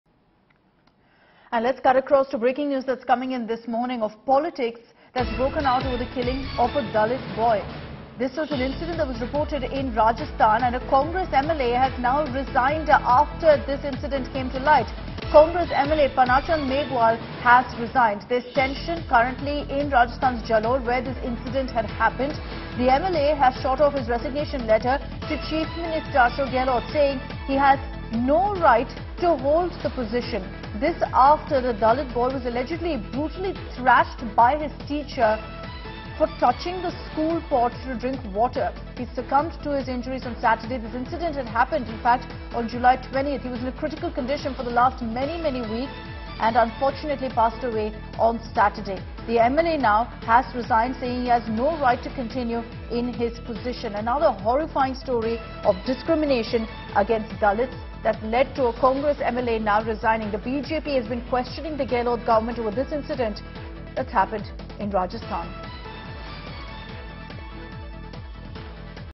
Congress MLA Panna Chand Resigns After Dalit Boy's Death; Tension Brews In Rajasthan's Jalore
A Congress lawmaker sent his resignation letter to chief minister Ashok Gehlot over the death of a 9-year-old Dalit boy who was allegedly thrashed by a teacher at school for touching a drinking water pot, saying atrocities against Dalits have been on the rise over the past few years and that he didn’t have the right to remain a legislator if he can’t protect the community.